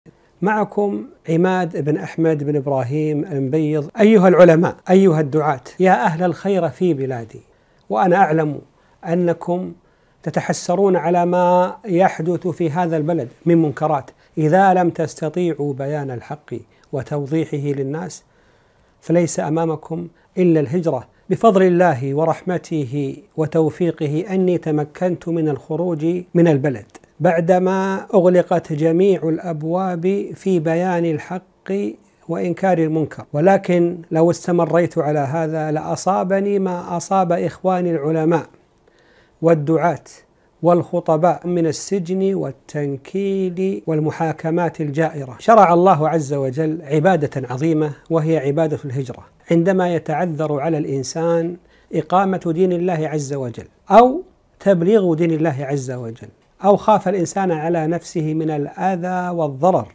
Saudi cleric who criticised the government flees the country
In a recent video posted to Twitter, Emad al-Moubayed, a Saudi Arabian cleric, said he had left the country for a safe location.

This comes after concerns were raised regarding his safety after he previously tweeted a video criticising recent reforms in the kingdom and advised those in power to "fear God".

In his recent video, he expressed the wisdom behind leaving the country and encouraged other clerics and activists who want to speak out against the Saudi government to do the same.

Many Islamic clerics are against the changes that have been happening in the kingdom in recent years but Saudi Arabia has been using cybercrime laws to muffle political dissent and silence critics.